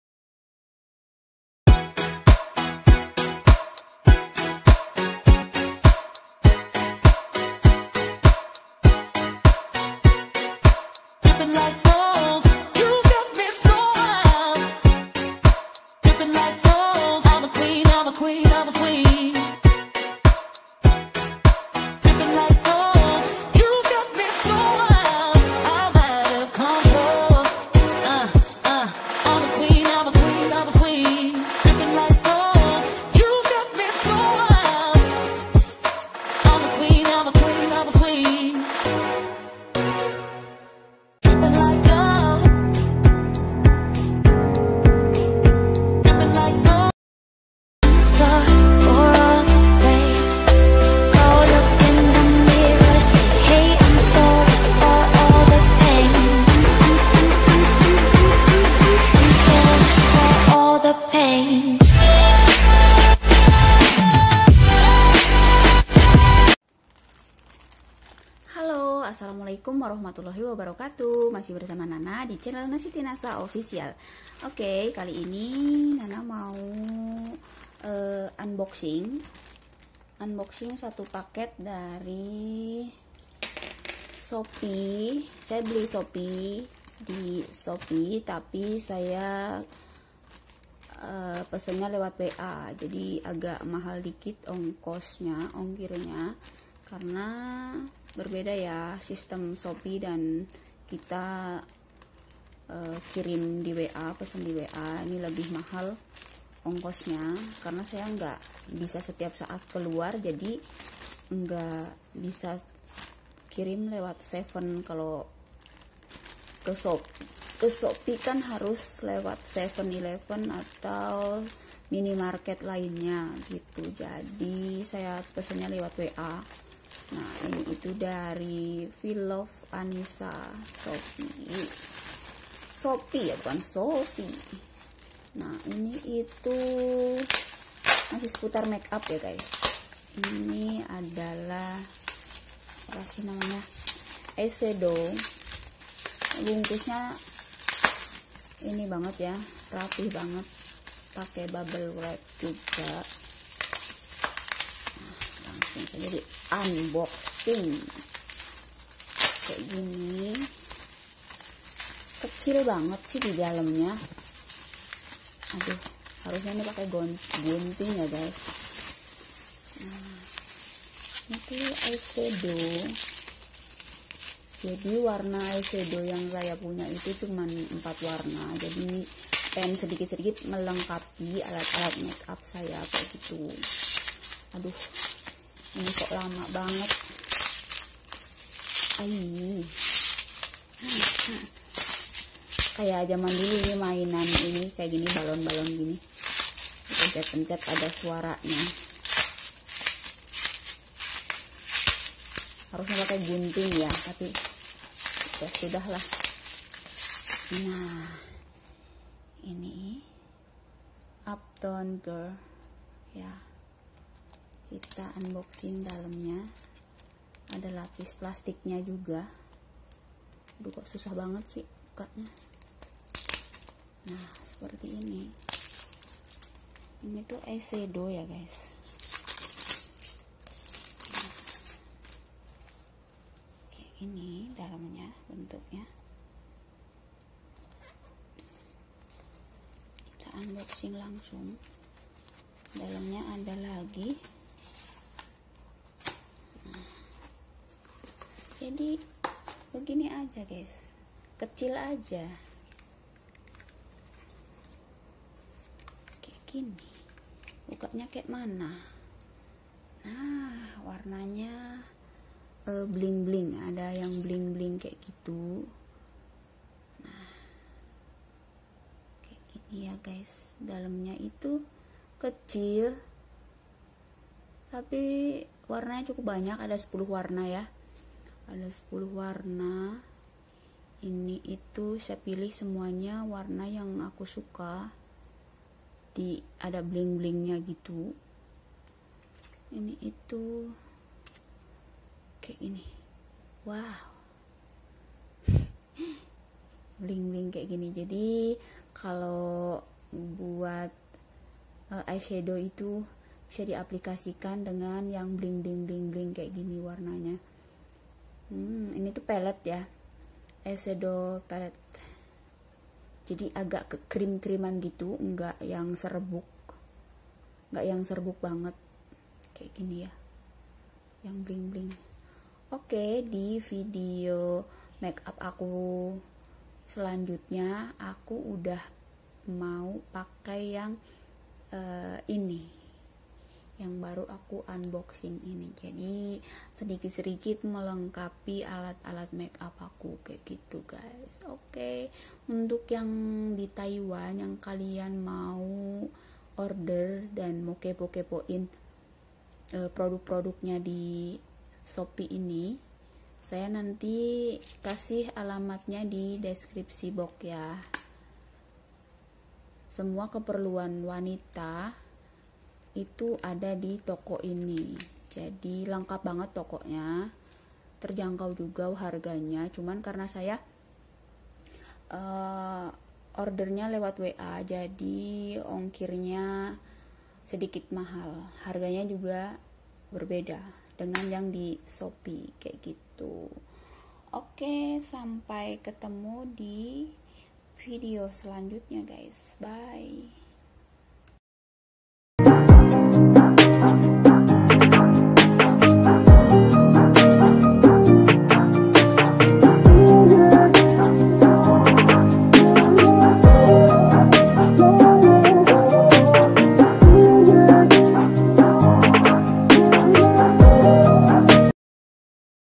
UNBOXING EYESHADOW PALETTE TERB4RU 2021 || SHOPEE TAIWAN ()
Dalam video kali ini Nana mau unboxing lagi nih guys
Masih seputar alat makeup yah
Kali ini mau sedikit melengkapi alat makeup q ,
Q beli Eyeshadow pallete

Untuk kalian yg pngin kepo in toko shopee nya link ada di sini 👇

Yuck ikuti q unboxing guys... 🥰